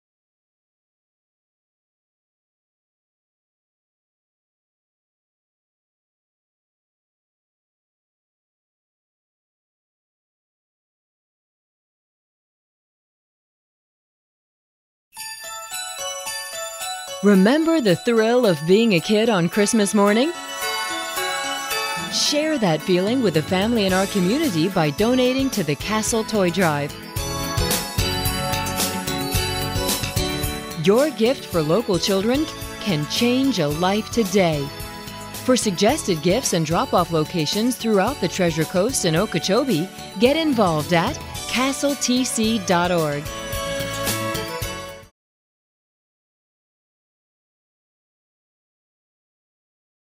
CASTLE TOY DRIVE1X30.avi
CASTLE (SAFE PARENTING AND PREVENTING CHILD ABUSE - Annual toy drive :30 seconds TV spot in south florida; treasure coast.
Producer:/editor: Dean Sfikas